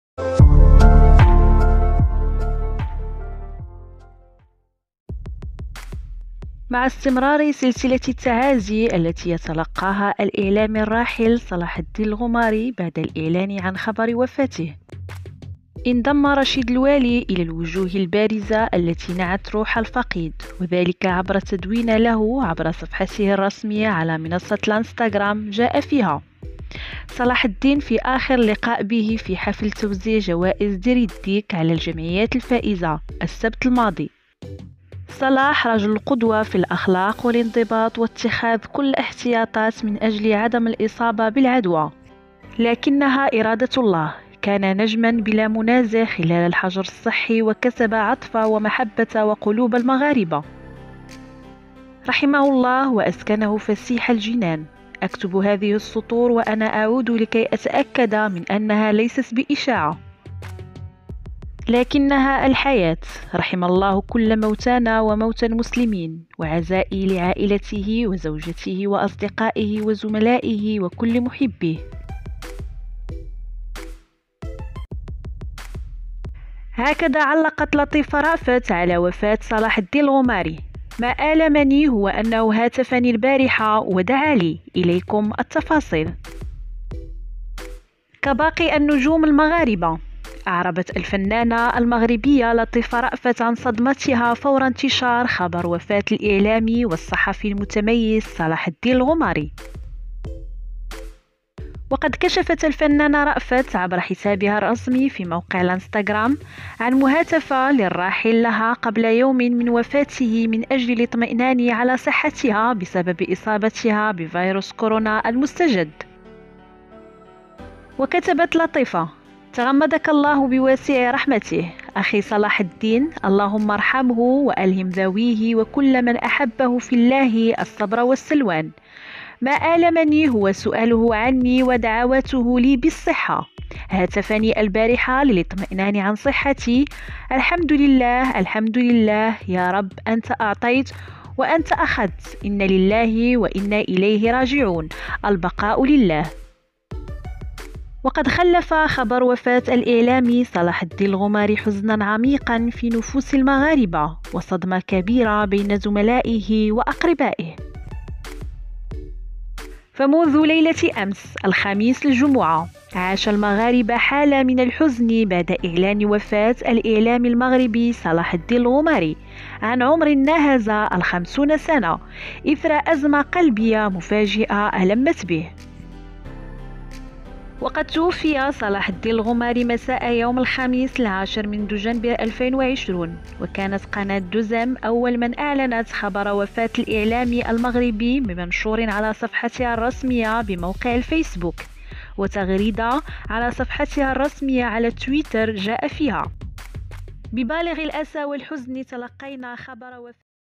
بالدموع رشيد الوالي يحكي تفاصيل آخر لقاءه مع الإعلامي قبل وداعه
بالد. موع رشيد الوالي يحكي تفاصيل آخر لقاءه مع الإعلامي  قبل وداعه